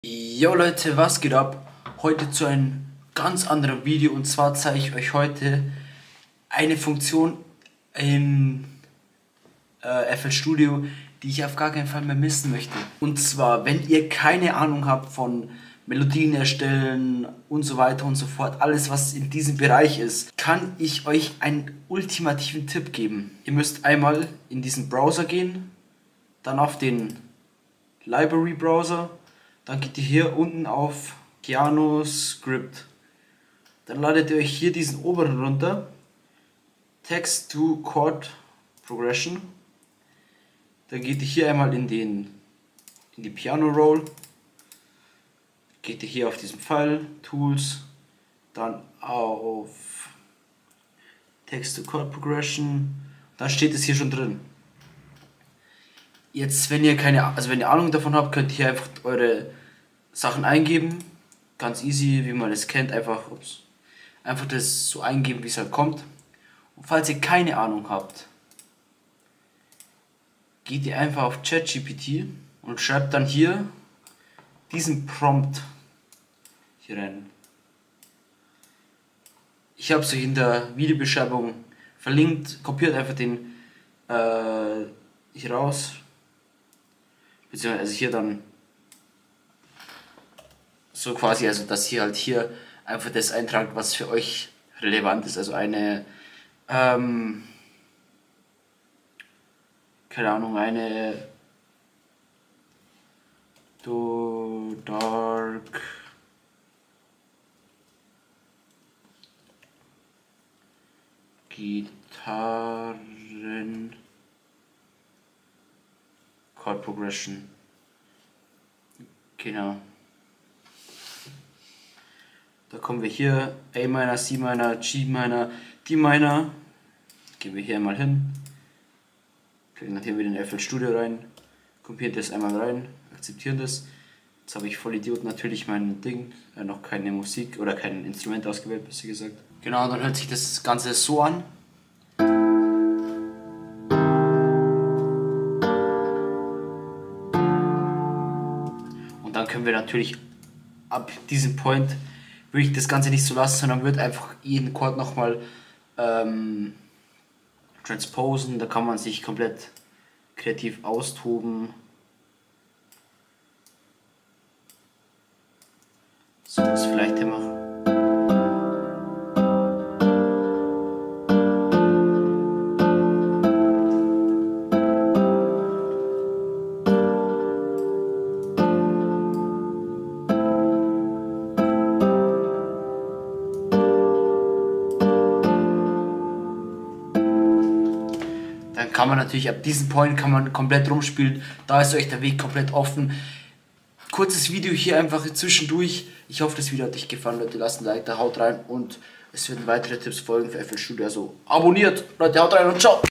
Wie ihr Melodien macht wie Metro Boomin!
▼Aufklappen ▼

Prompt:
Hey ChatGPT schreib mir eine [Beschreibung] Chord Progression. Schreibe bitte jeden Chord neben den anderen mit einen Komma dazwischen.